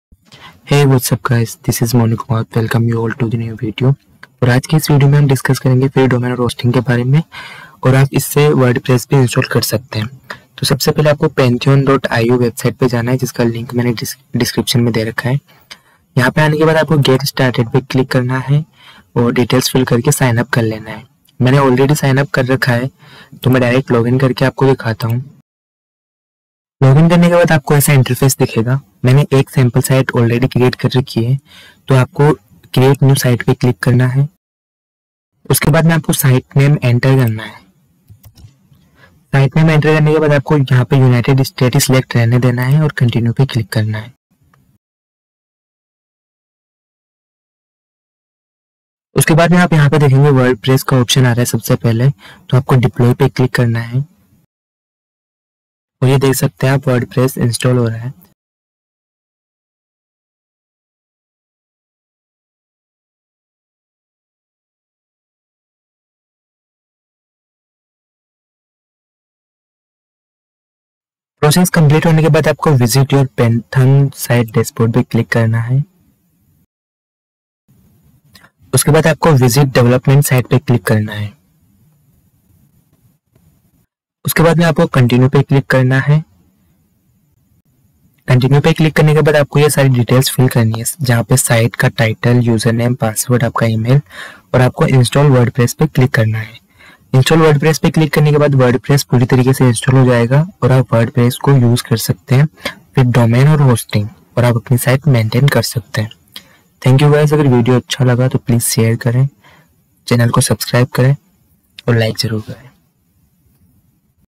Lifetime Free Hosting + Free Domain + Wordpress With cPanel Companies 2021
if You are Looking For Lifetime Unlimited Hosting FOR FREE With Free Domain, Free SSL, Free cPanel on WordPress then this is the best Video for You.